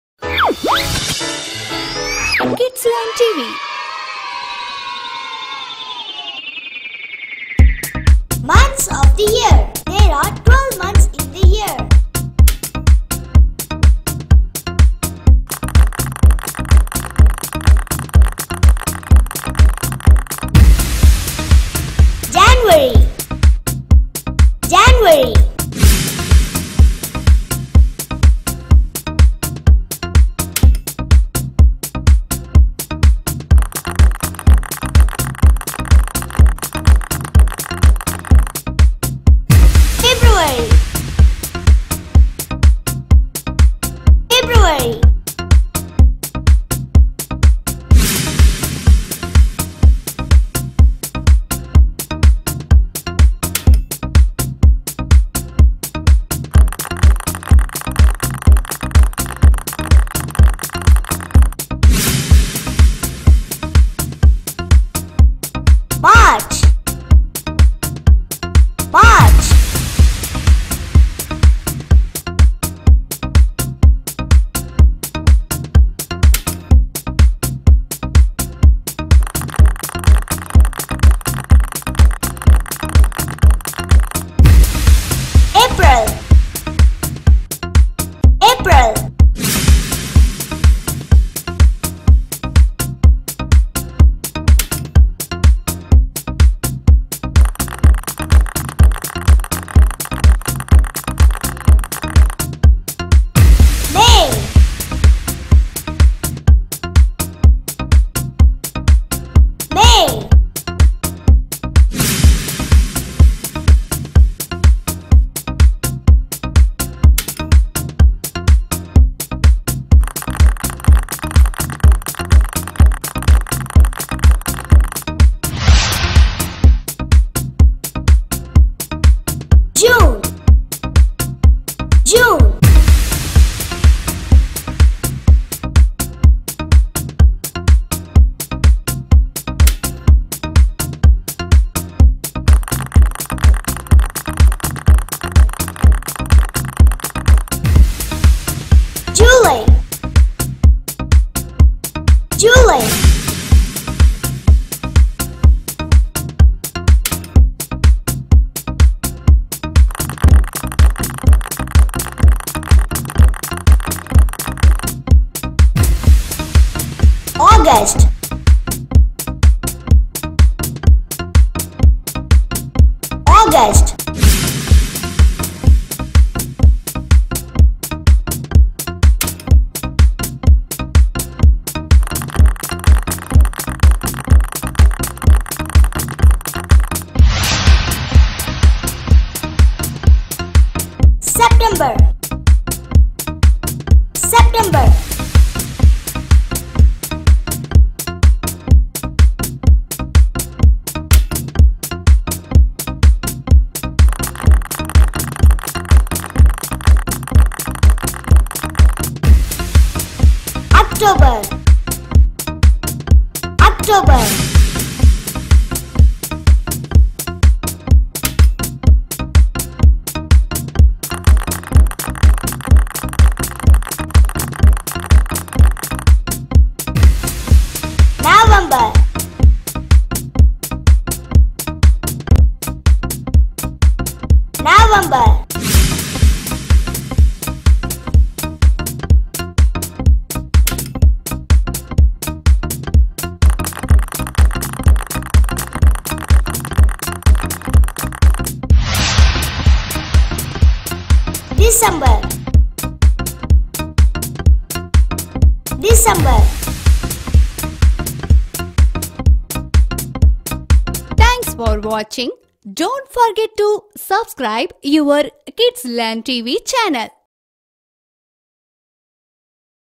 Name of months - Learn Month name in English - Months of the year for kids
Name of months video to teach Twelve months names January, February, March, April, May, June, July, August, September, October, November, December

Designed by :
                    Jongwings studios

                   BM Studio - Madurai .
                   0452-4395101